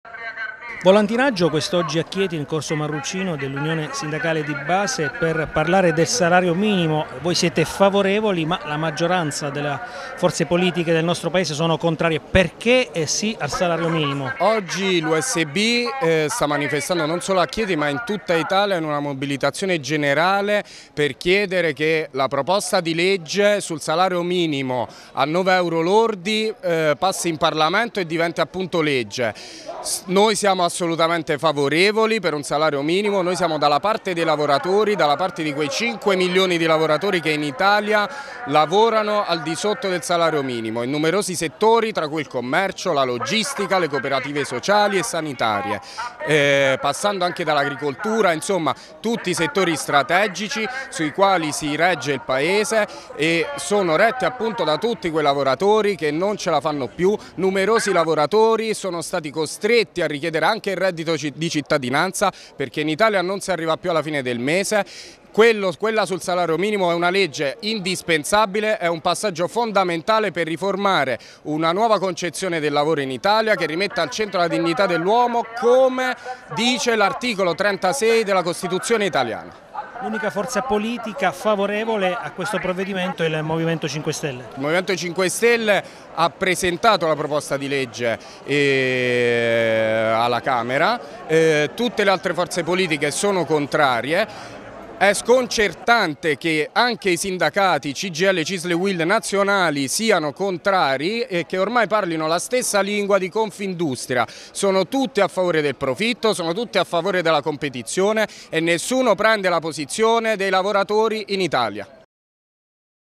L'Unione Sindacale di Base manifesta per il salario minimo
iniziativa pubblica di denuncia sociale e di sensibilizzazione, nella giornata di mobilitazione nazionale del sindacato Unione Sindacale di Base in favore del Salario Minimo contro il Lavoro Povero. L'USB Abruzzo ha manifestato questa mattina a Chieti con un volantinaggio